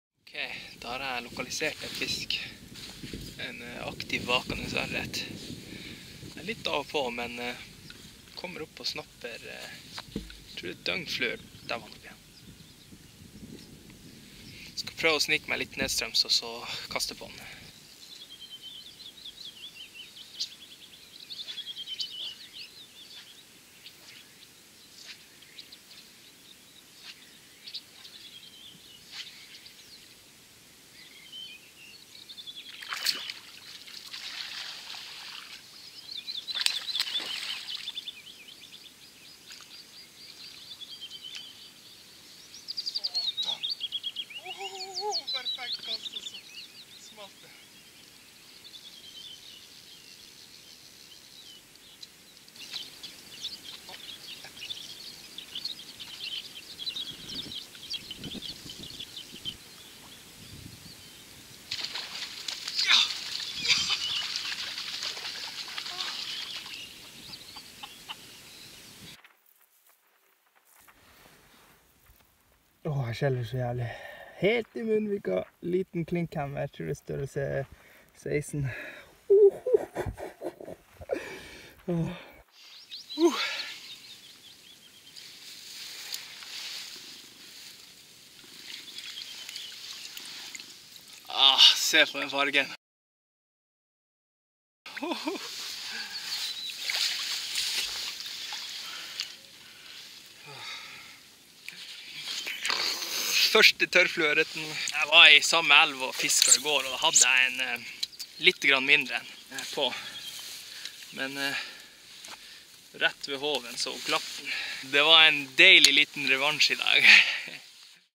En kort en om den første.
Har endelig fått fisket litt med 5# stanga, og at jeg kun har fisket litt med fluestenger bærer kastene mine preg av. Har tatt et par harr på tørt, men har ikke fått prøvd meg skikkelig på ørreten tidligere.
Så da dro jeg til en elv hvor jeg vet det svømmer noen store individer for å prøve meg.
Observerte en fisk på 1-1,5kg første dagen, og hadde en litt mindre fisk på samme dagen. Den glapp dessverre. Prøvde meg igjen dagen etter, og da smalt denne skjønnheten på 8hekto. Fin første fisk på fluestanga.
Har en til video på vei ;)

Og følg meg gjerne også på instagram under navnet Arctic.predator.fishing